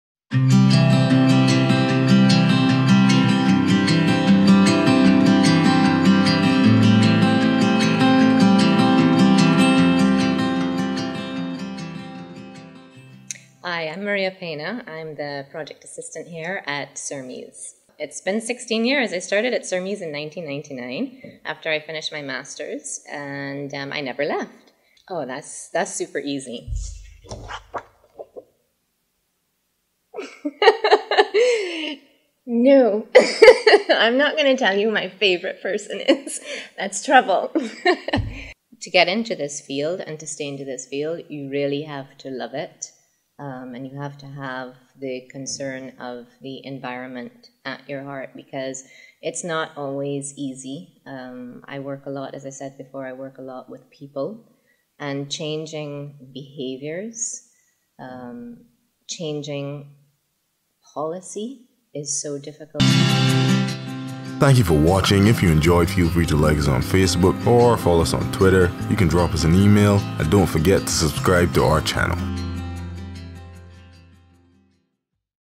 Meet Maria (Teaser)
Maria Pena’s interests are in the biological and socio-economic aspects of marine resource management. Before joining CERMES in 2001, she was Research Assistant with the Natural Resource Management Programme, UWI, Cave Hill Campus conducting genetic analyses on the white sea urchin, Tripneustes ventricosus, in the eastern Caribbean in an attempt to confirm the population stock structure of this species.

More recently, in the capacity of Project Assistant at CERMES, she has been involved in, assisted with, implemented and managed numerous externally funded marine resource management projects including socio-economic monitoring at marine protected areas (MPAs) and coastal sites (at many sites in the eastern Caribbean); fisheries management planning (at the Negril Marine Park, Jamaica); MPA management effectiveness evaluation (at the Negril Marine Park, Jamaica; Tobago Cays Marine Park, St. Vincent and the Grenadines and Sapodilla Cayes Marine Reserve, Belize); marine resource governance  (in the eastern Caribbean); and more recently climate change adaptation and disaster risk reduction in fisheries and aquaculture (in CARICOM). Her project work frequently includes stakeholder engagement and outreach.

Maria is currently the Regional SocMon Coordinator for the English-speaking Caribbean. Her experience with the Global Socio-economic Monitoring for Coastal Management (SocMon) initiative includes assisting with the inaugural SocMon Caribbean regional workshop in 2003; conducting training in support of and managing the Socio-economic Monitoring by Caribbean Fishery Authorities project (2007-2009); promoting SocMon at numerous marine training courses and conferences; and most recently, project management of the NFWF-funded Socio-economic Monitoring by Caribbean Challenge MPA managers project (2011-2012) and NOAA Coral Reef Conservation Program funded Socio-economic Monitoring at
National Marine parks in the Turks and Caicos Islands (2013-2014).

Maria has been a part-time lecturer at the UWI from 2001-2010.

Recent Publications:
Pena M., K. Blackman and P. McConney. 2013. Final Technical Report. Socio-economic Monitoring by Caribbean Challenge MPA Managers Project Report No. 11. 64 pp.
Availability: request from author

Pena, M., P. McConney, and K. Blackman. 2012. Common Socio-economic monitoring indicators for Caribbean Challenge MPAs. Proceedings of the Gulf and Caribbean Fisheries Institute, 65:193-203.

McConney P. and M. Pena. 2012 Capacity for (Co)Management of Marine Protected Areas in the Caribbean. Coastal Management, 40:3, 268-278.

Pena, M. and P .McConney. 2010. Institutional arrangements for local management of marine areas in the eastern Caribbean. Proceedings of the Gulf and Caribbean Fisheries Institute 63: 267-273.

Pena, M., H.A. Oxenford, C. Parker and A. Johnson.2010. Biology and fishery management of the white sea urchin, Tripneustes ventricosus, in the eastern Caribbean. FAO Fisheries and Aquaculture Circular # 1056, 43 pp.

Climate Change,Adaptation, Environmental & Natural Resource Management are essential for Sustainable Development in the Caribbean Context!